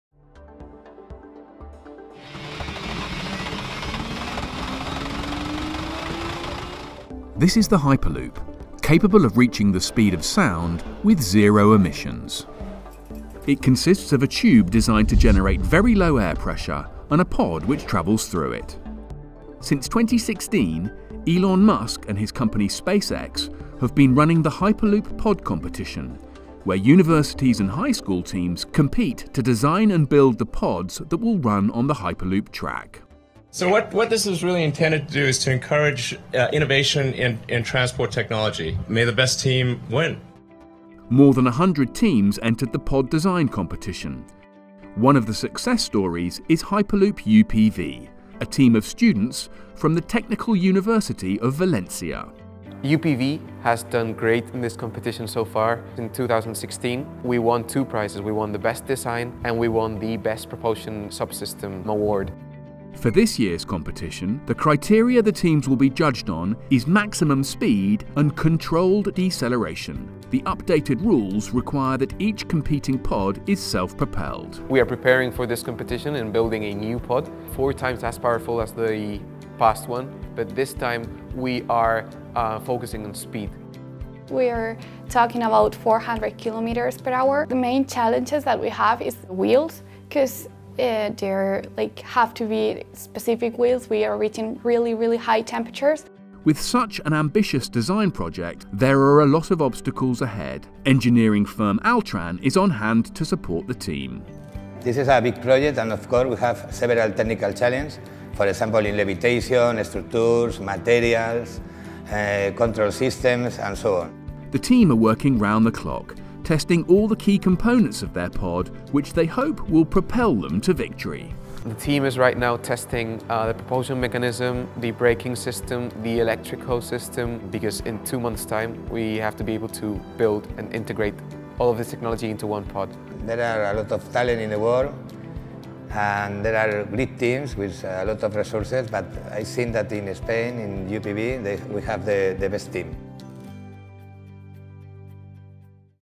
Paid Post - Hyperloop: designing the future of transport? | Industrial Tech
Elon Musk hopes to revolutionise public transport with Hyperloop - a system capable of speeds of more than 1200 kilometres per hour with zero emissions. Musk’s company Space X is running a student competition to advance the technology. Hyperloop UPV, a team from the Technical University of Valencia in Spain, are working round the clock testing all the key components of their pod, which they hope will propel them to victory.